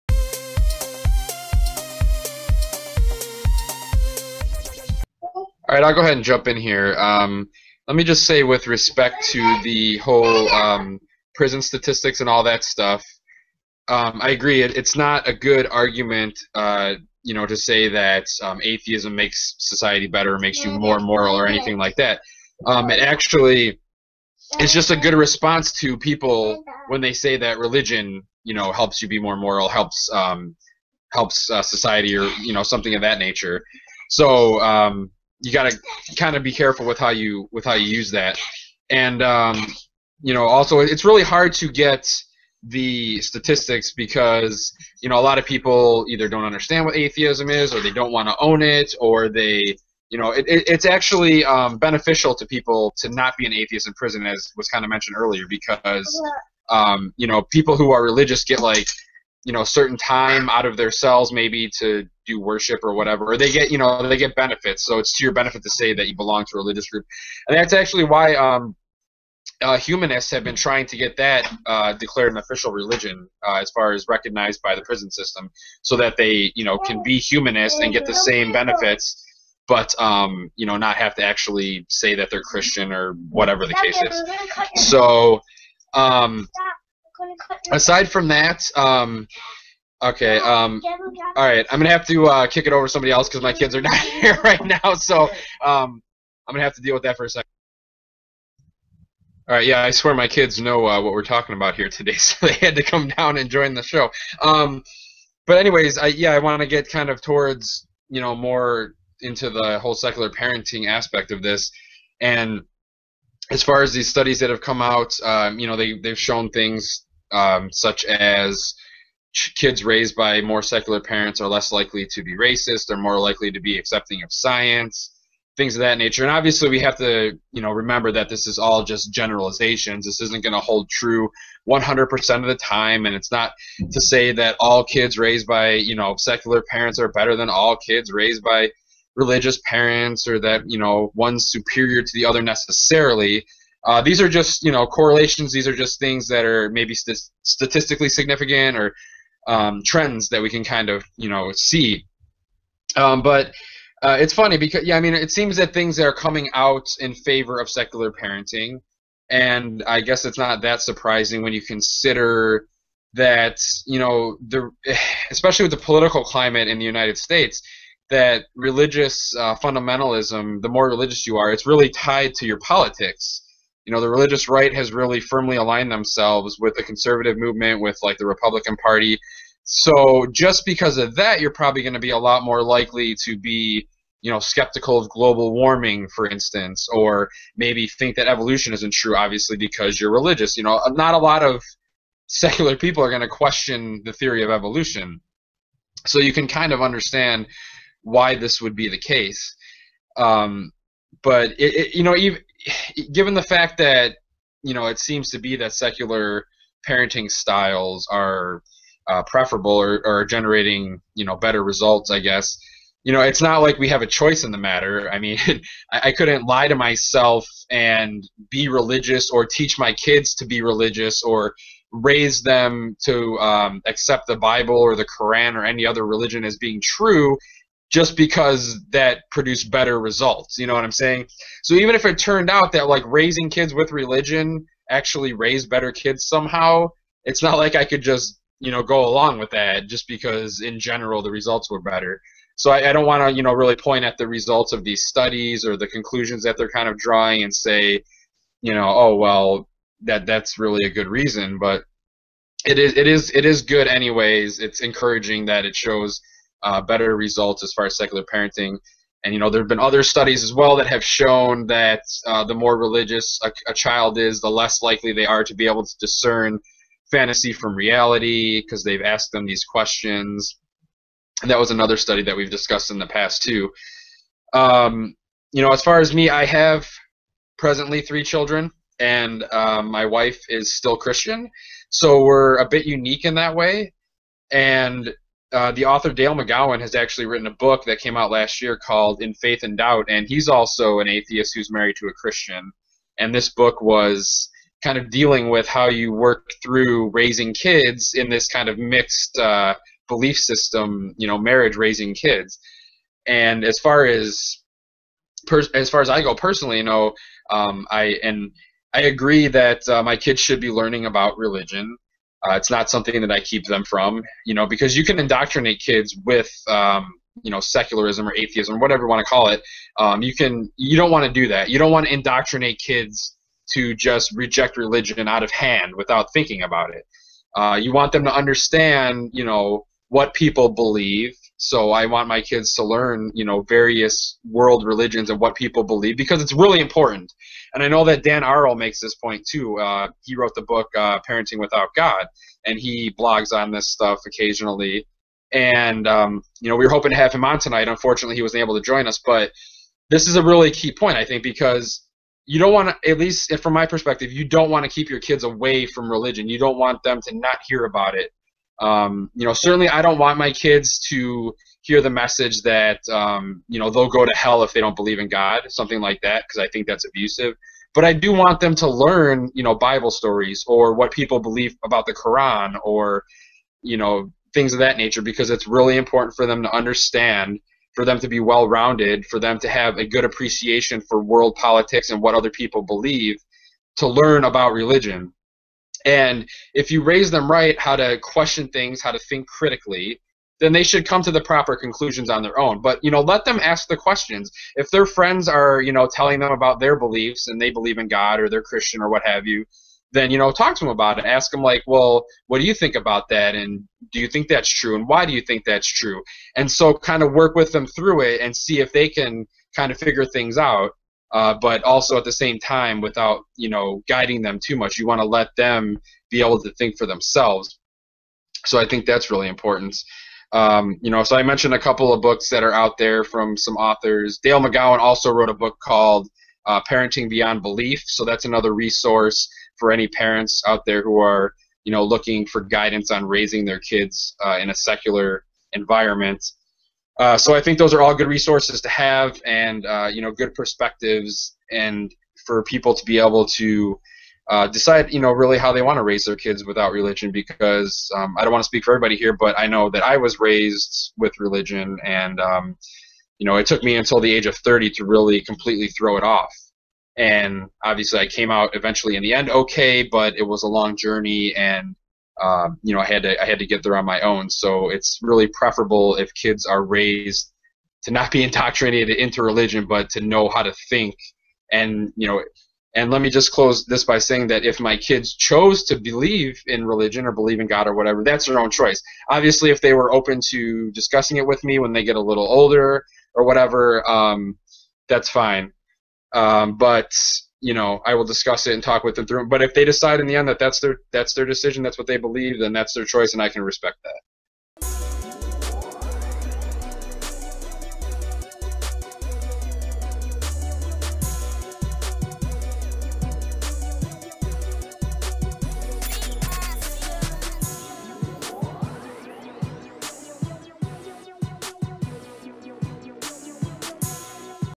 REASON #20: Secular Parenting
Clip from the 2/15/15 episode of the REASON (Religiously Educated Atheists Secularists and Other Non-theists) secular roundtable discussion regarding secular parenting.

Please join us live each Sunday at 9pm Eastern for the REASON discussion panel, broadcast at YouTube channel: